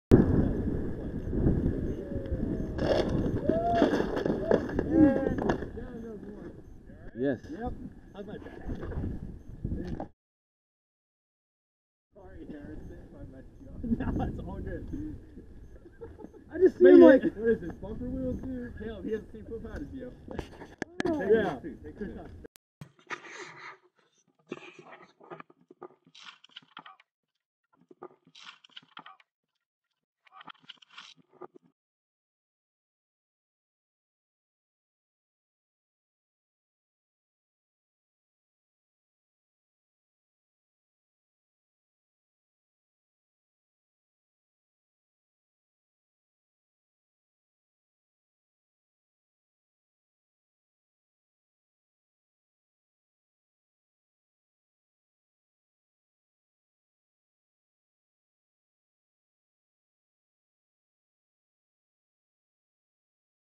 Harrison Falls in Beaver
On 3/31/2022 a group of six of us were riding around in the town of Beaver.  Harrison is normally an adept rider but not so much on this day.
Wrist guards and elbow pads aren't just for the trails.

The first part of the video was captured with the Maxto M3 helmet camera and the second was captured with the Rylo 360 and edited in the Insta360 Studio software.